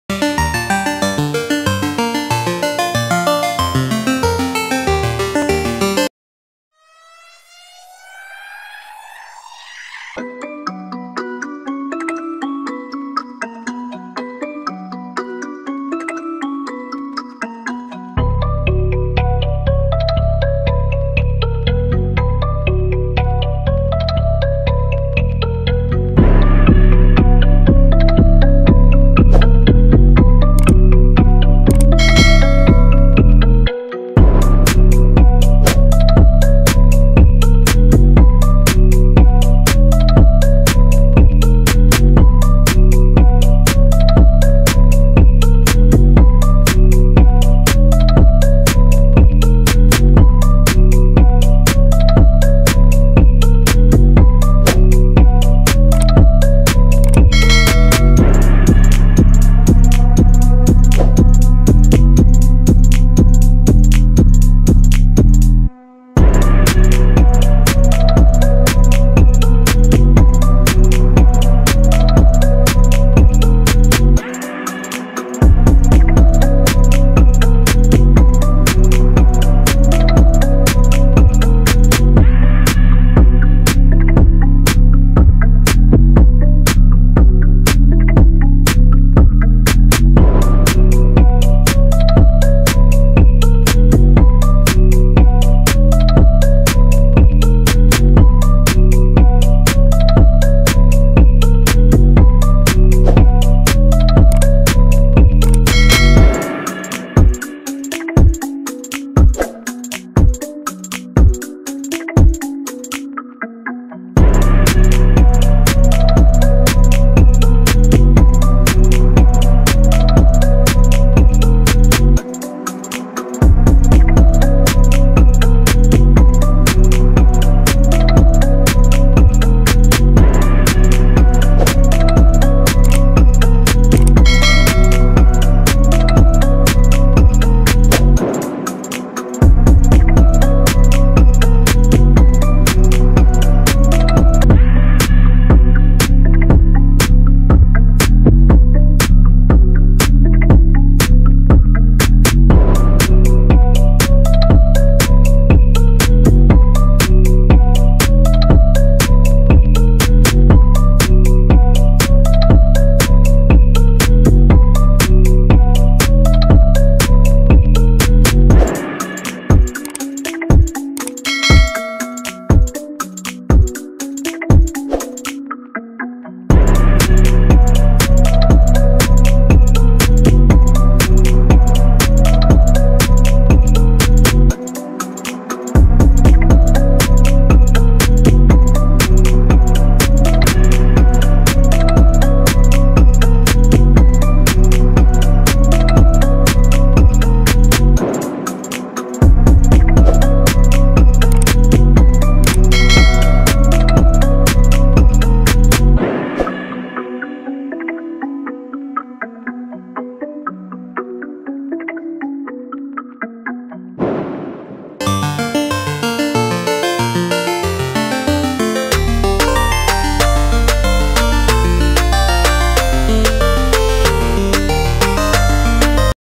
Draw Rakhi with letter S //Step by step guide//
Learn to draw Rakhi with letter S.
Step by step guide to draw Rakhi.